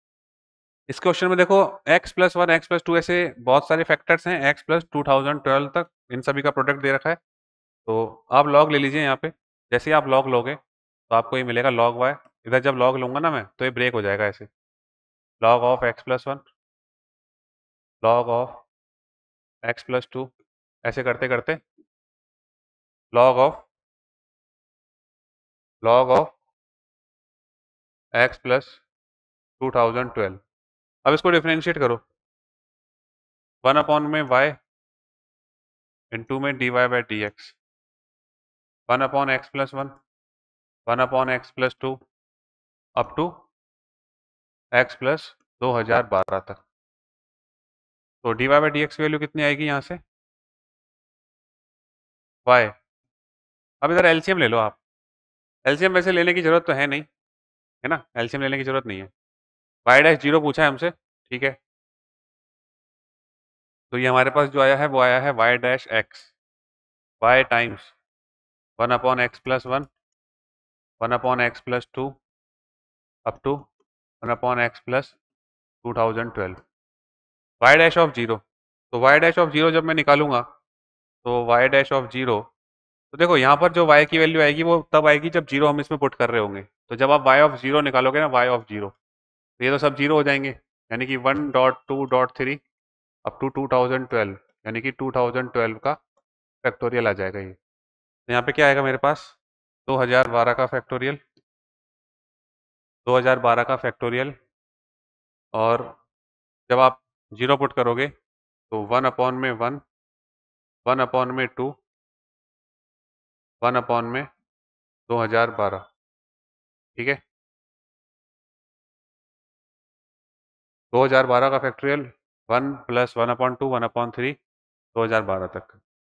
If y = (x + 1) (x + 2) (x + 3) &hellip; (x + 2012), then the value of y&rsquo;(0) is:&nbsp;....
If y = (x + 1) (x + 2) (x + 3) &hellip; (x + 2012), then the value of y&rsquo;(0) is:&nbsp; 📲PW App